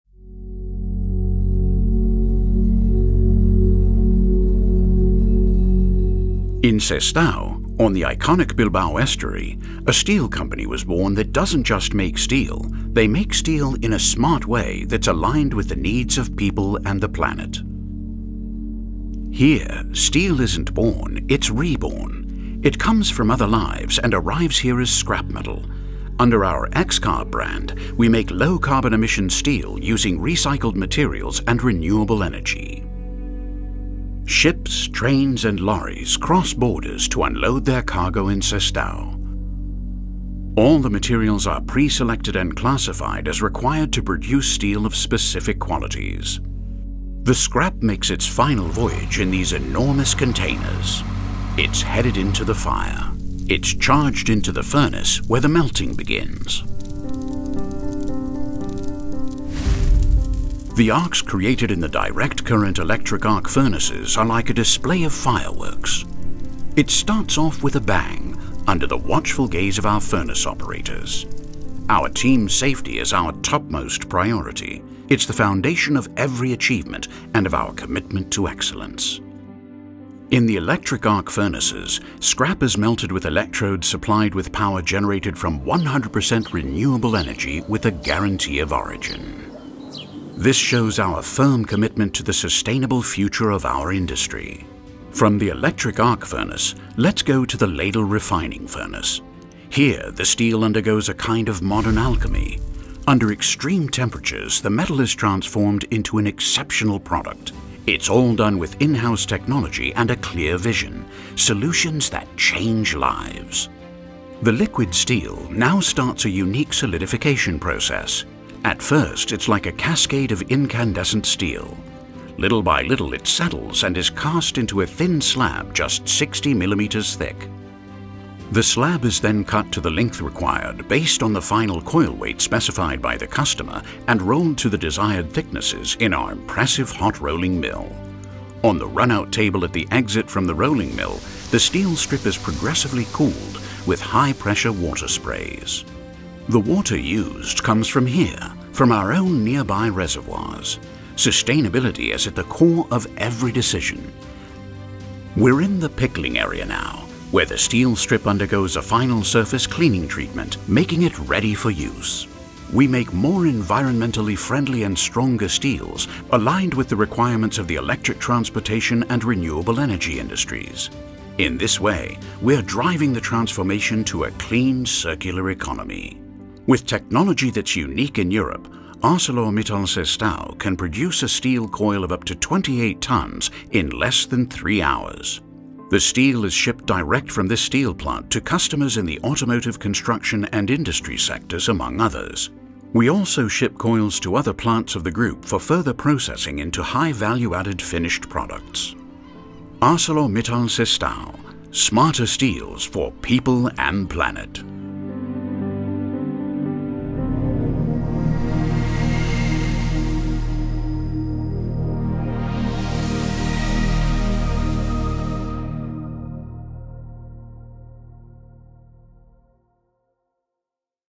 ArcelorMittal Sestao (ENG)
ArcelorMittal Europe – Flat Products’ Sestao site in northern Spain is playing a key role in our company’s decarbonisation strategy, as the first Flat Products site in the group to produce XCarb® recycled and renewably produced steel, through Electric Arc Furnace (EAF) technology.